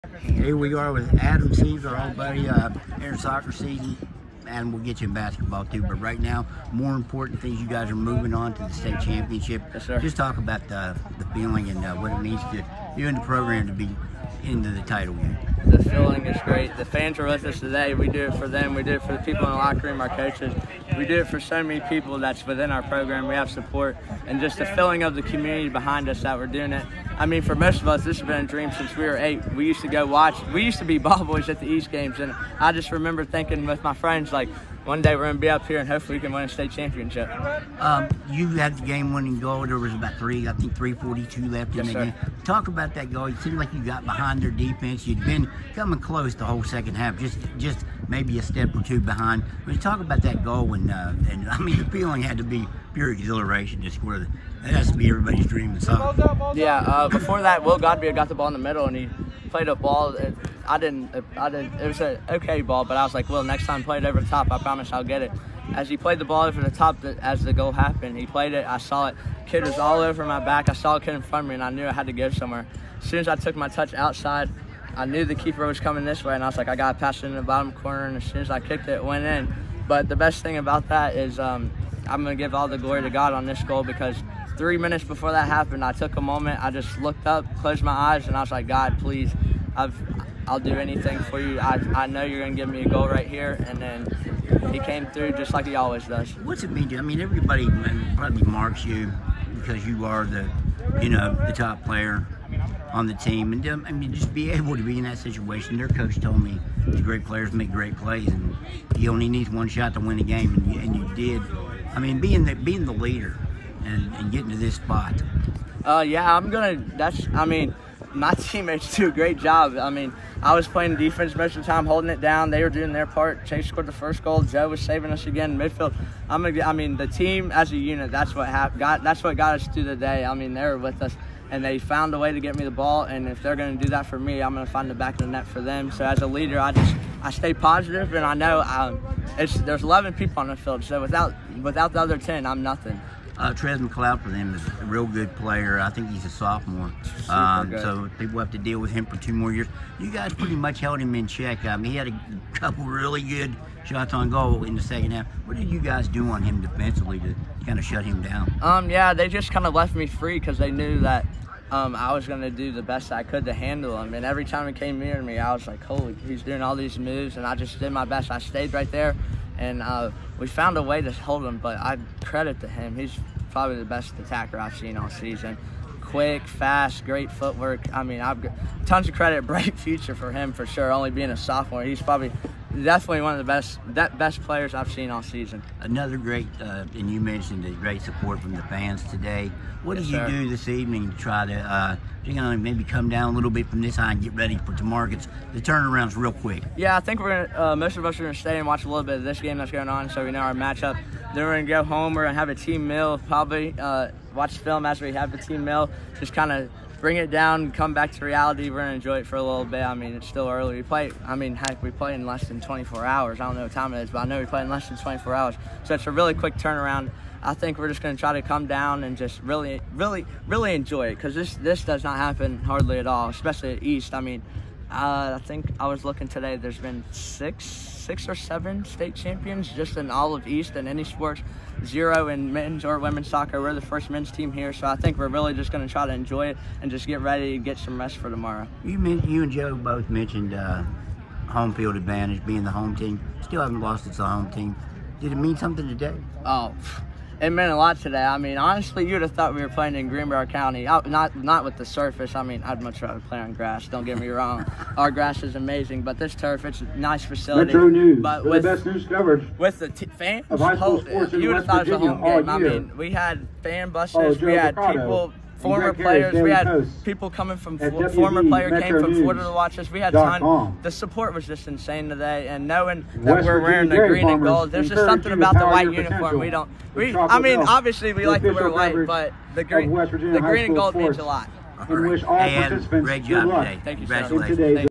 Greenbrier East’s Adam Seams
Greenbrier East’s Adam Seams on game-winning goal, moving on to state championship game and a key move after 2-1 state tournament semifinal win against Wheeling Park Friday in Beckley.